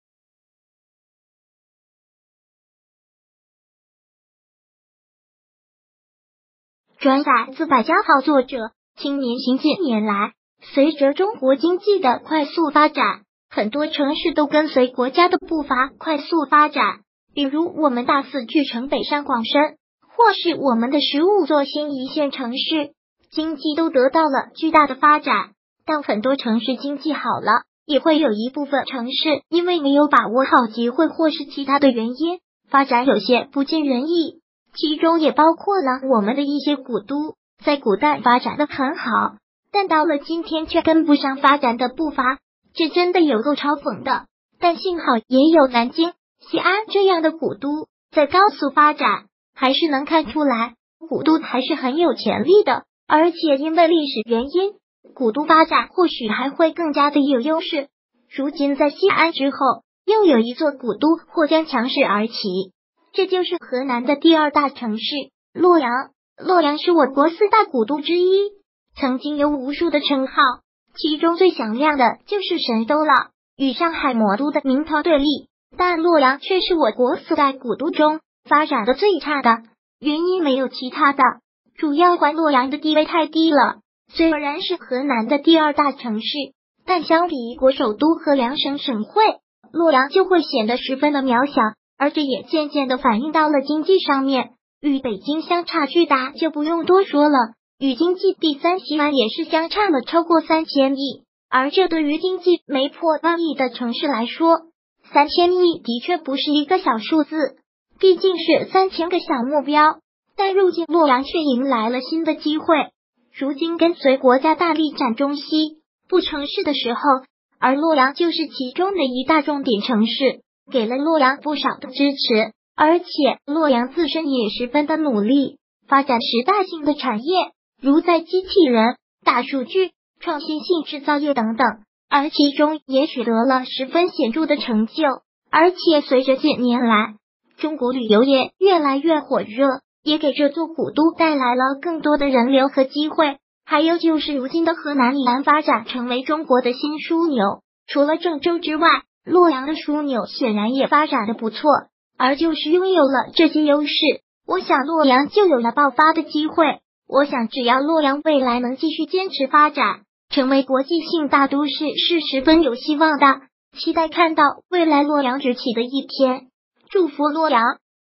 下一个西安！又一座古都重现活力，未来有望竞争国际大都市
转载自百家号作者：青年行近年来，随着中国经济的快速发展，很多城市都跟随国家的步伐快速发展，比如我们大四巨城北上广深，或是我们的十五座新一线城市，经济都得到了巨大的发展，但很多城市经济好了，也会有一部分城市因为没有把握好机会或是其他的原因，发展有些不尽人意，其中也包括了我们的一些古都。在古代发展得很好，但到了今天却跟不上发展的步伐，这真的有够嘲讽的，但幸好也有南京、西安这样的古都，在高速发展，还是能看出来，古都还是很有潜力的，而且因为历史原因，古都发展或许还会更加的有优势，如今在西安之后，又有一座古都或将强势而起，这就是河南的第二大城市，洛阳！洛阳是我国四大古都之一，曾经有无数的称号，其中最响亮的就是神都了，与上海魔都的名头对立。但洛阳却是我国四大古都中，发展得最差的，原因没有其他的，主要还洛阳的地位太低了，虽然是河南的第二大城市，但相比一国首都和两省省会，洛阳就会显得十分的渺小，而这也渐渐的反映到了经济上面，与北京相差巨大就不用多说了，与经济第三西安也是相差了超过3000亿，而这对于经济没破万亿的城市来说，3000亿的确不是一个小数字，毕竟是3000个小目标，但入境洛阳却迎来了新的机会